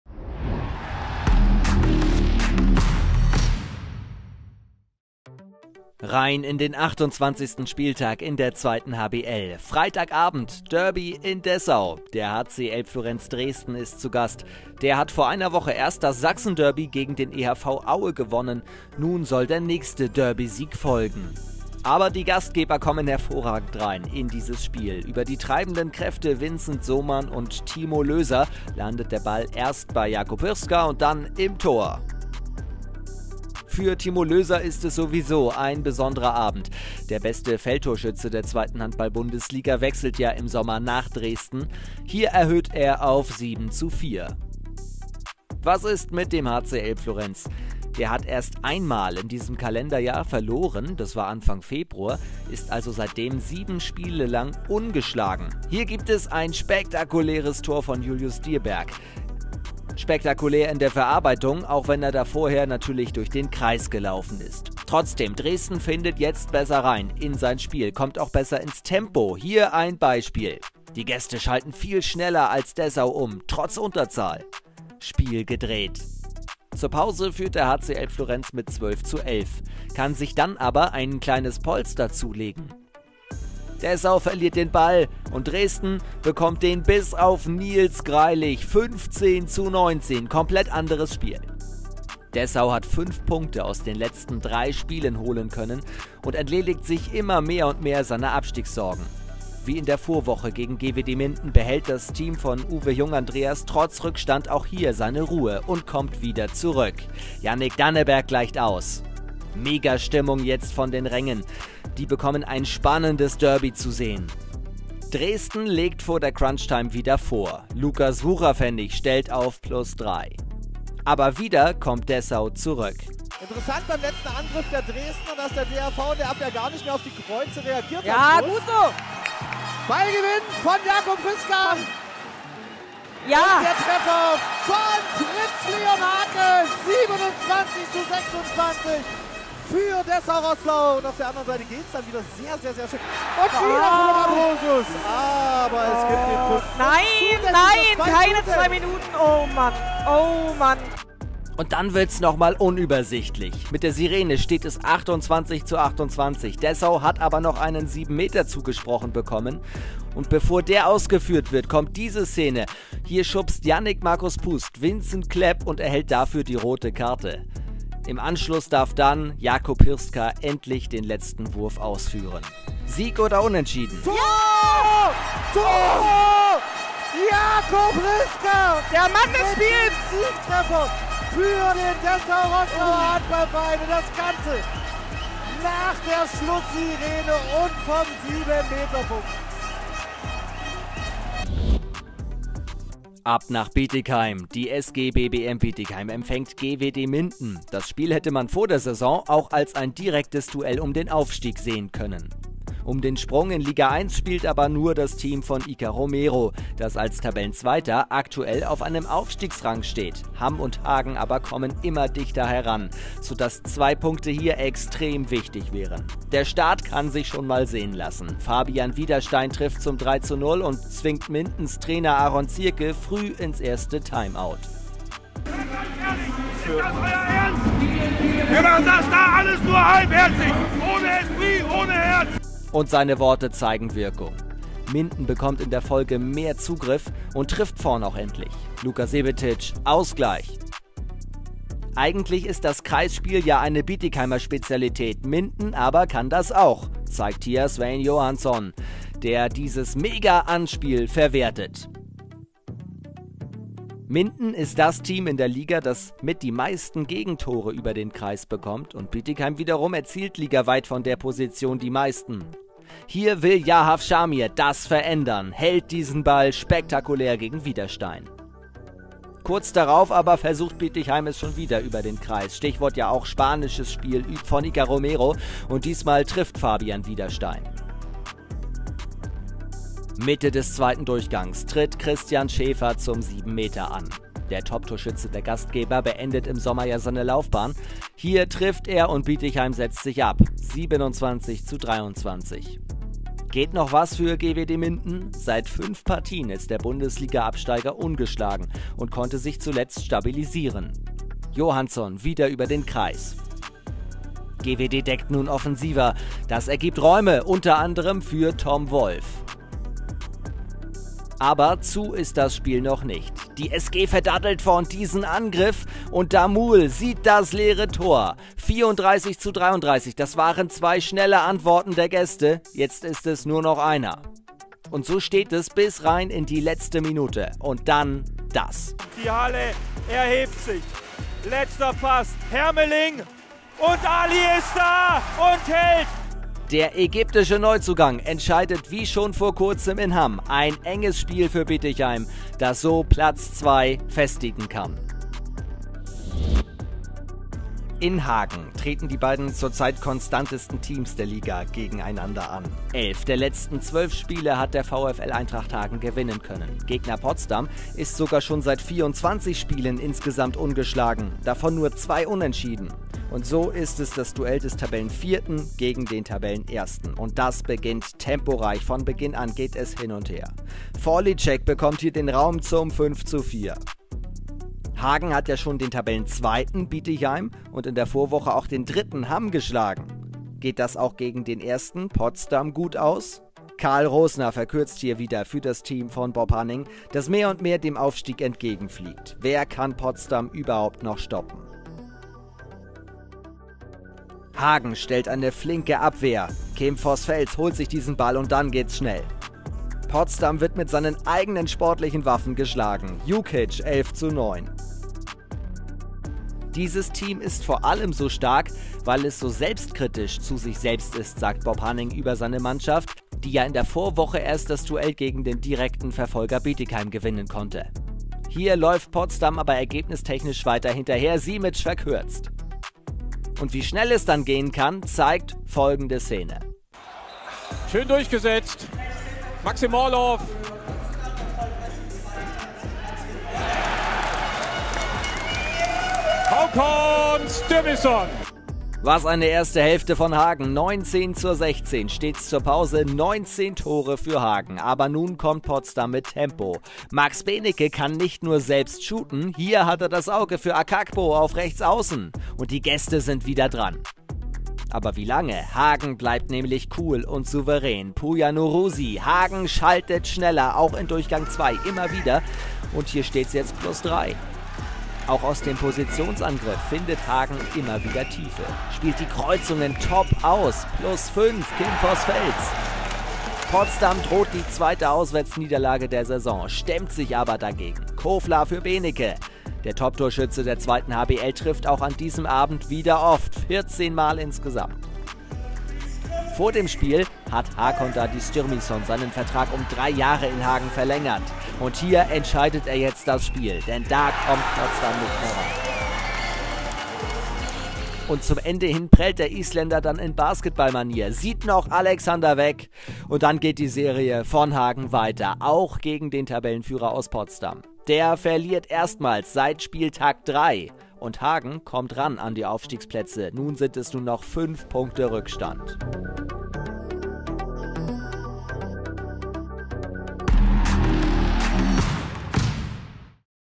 28. Spieltag in der 2. HBL | Die Highlights im Dyn Flashback
Im Flashback 2. HBL von Dyn präsentieren wir euch die Highlights des 28. Spieltags!

Jede Woche neu bekommt ihr hier zu jedem Spieltag alle Highlights und die besten Szenen der 2. Handball-Bundesliga.

00:00 - 03:10 Dessau-Rosslauer HV 06 vs. HC Elbflorenz 2006
03:11 - 05:46 SG BBM Bietigheim vs. GWD Minden
05:47 - 08:44 VfL Eintracht Hagen vs. 1. VfL Potsdam

Alle Spiele der 2. HBL live und auf Abruf bei Dyn Handball